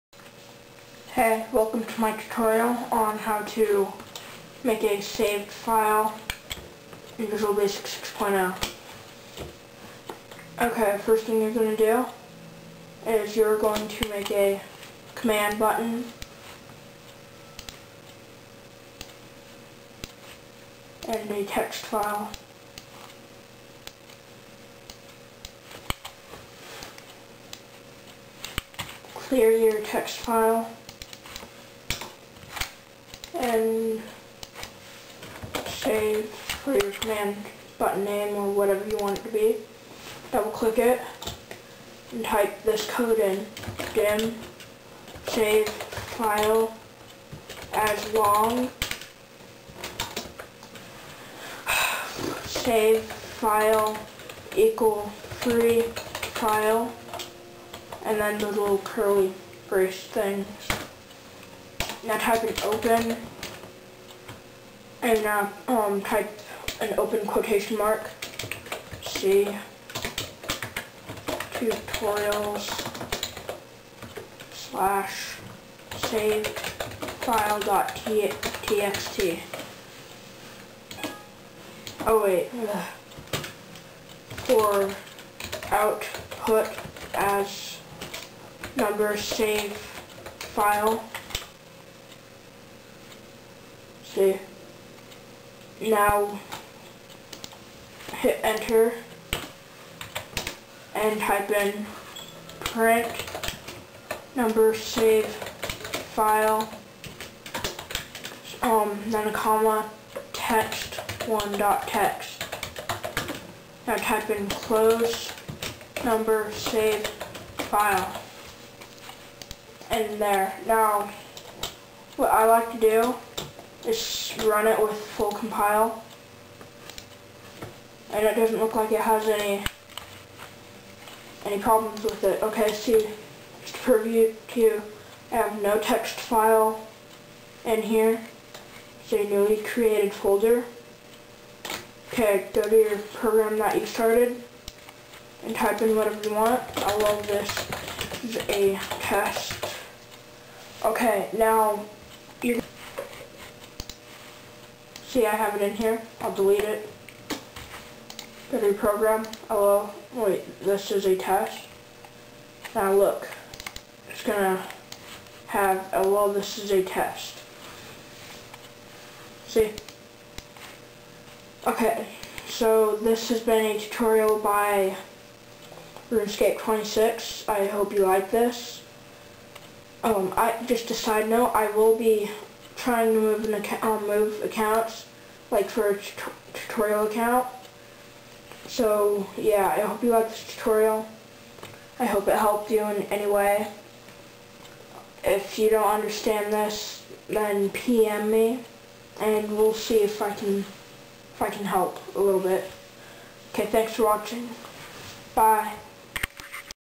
Save a file anywhere (Visual basic)
Hey, this is how to save a file anywhere, if you don't understand the code, or don't see it, because youtube raped the quality, this is it

Private Sub Command1_Click()
Dim Savefile As Long
Savefile = FreeFile()
Open "C:\Test\savedfile.txt" For Output As
Print Savefile, Text1.Text
Close
End Sub

Thanks Kaje (I think it is)

Thats the code ^^
Well, hope this helps you.